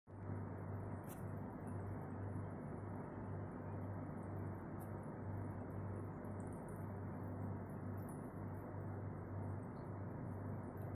tiny goby !
Tiny goby from mediterranean sea!
About 1 cm length, not problem with anemones and shrimps
moves very quicly!
More videos comming soon - how to catch fishes for aquarium